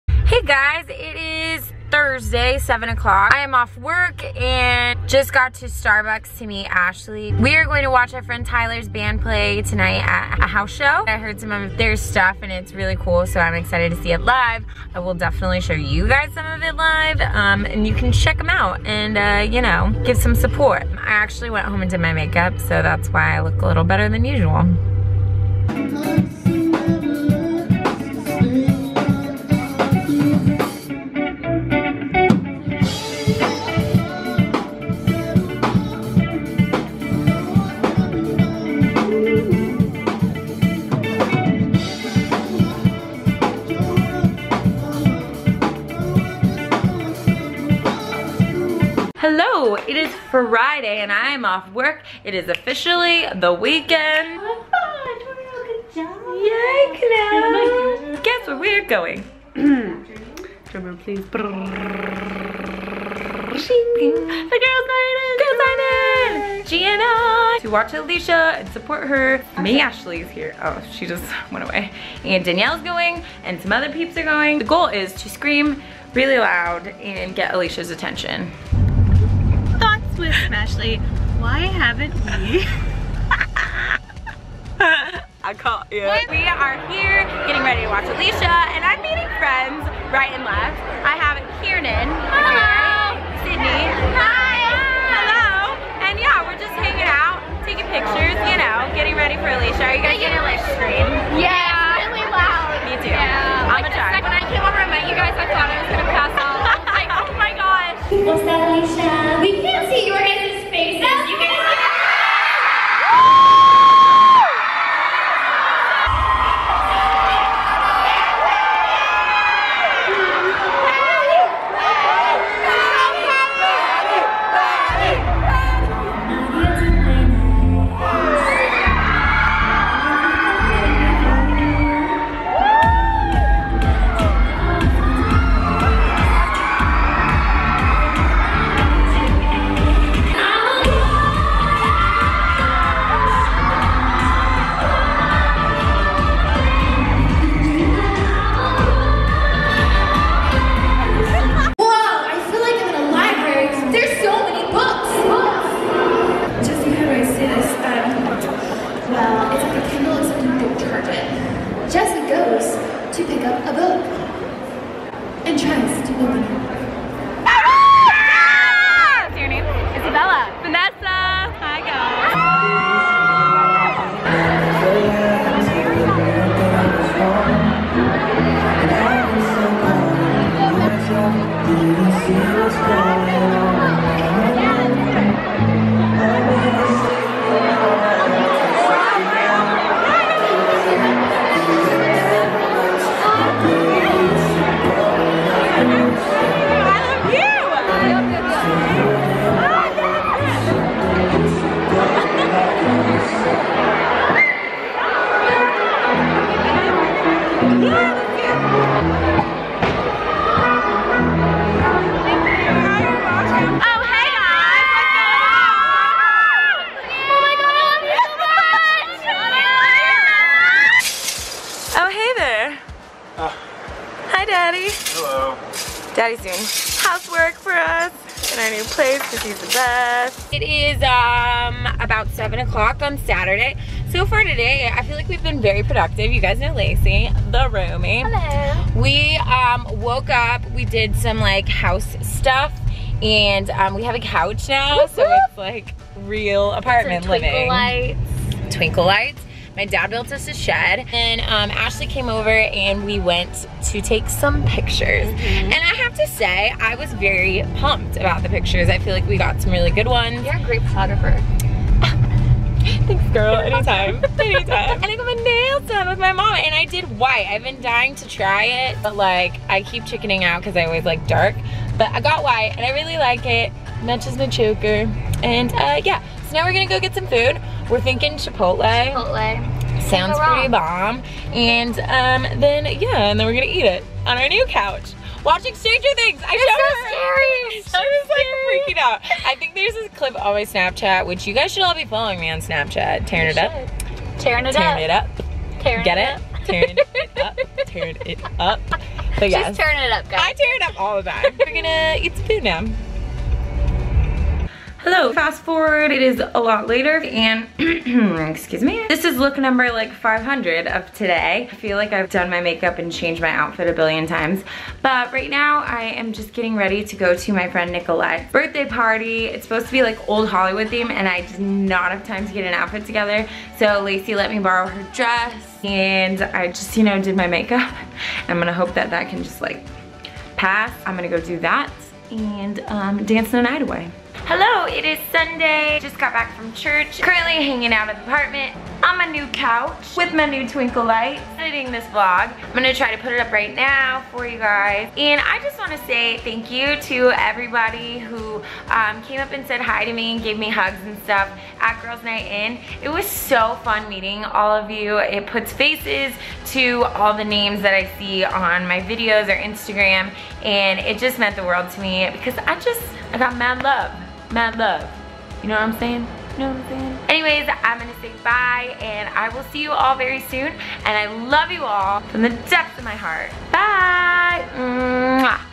Girls Night In Tour!!! :)
Got to go watch my boo thang Alisha Marie kill it at Girls Night In!! Soooo proud!!